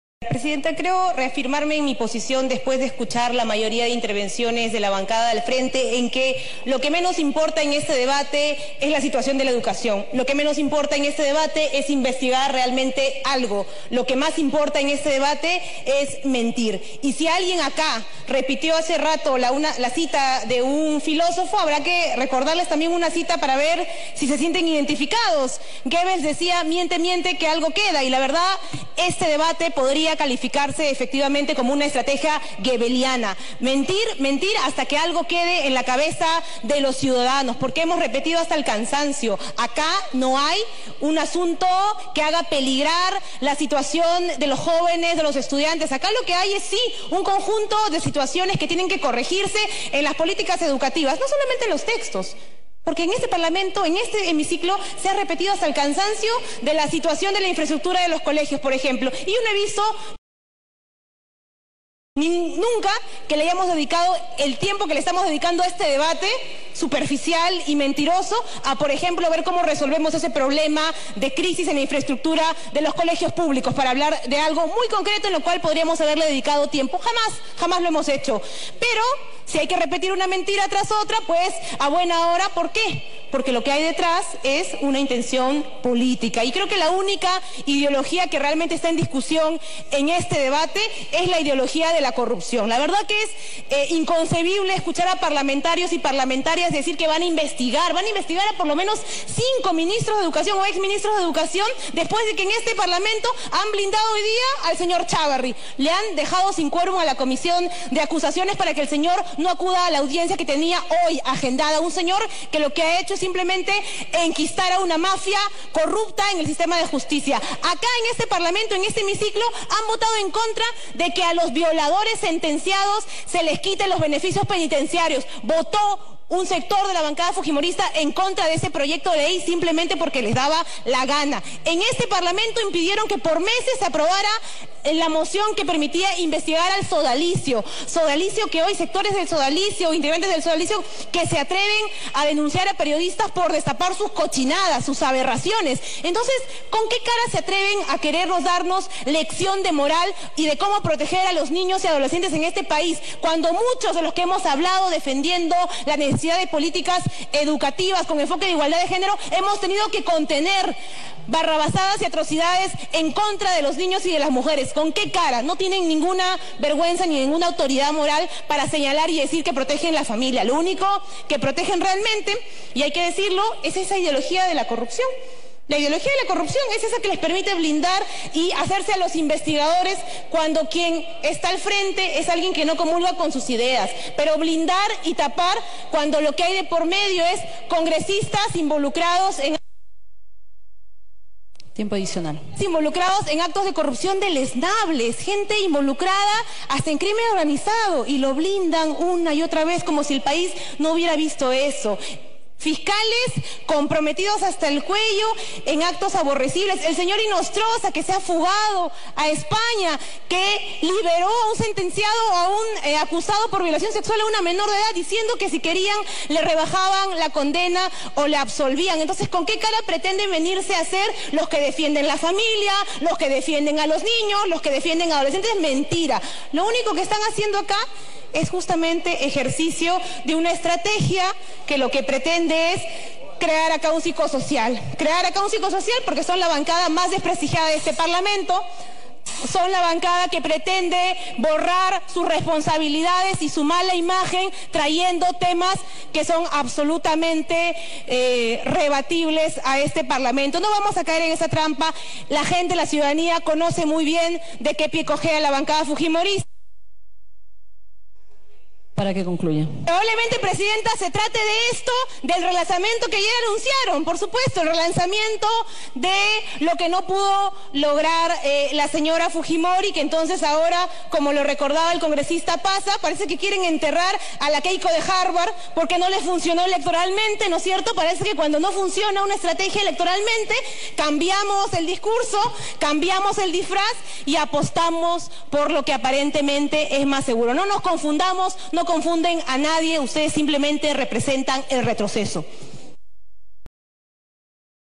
🔴PERÚ: CONGRESISTA HINDIRA HUILCA LE CANTA SUS VERDADES AL FUJIMORISMO
IMPLACABLE. La congresista de NuevoPerú HindiraHuilca denunció este martes en Congreso, el doble rasero moral del Fujimorismo.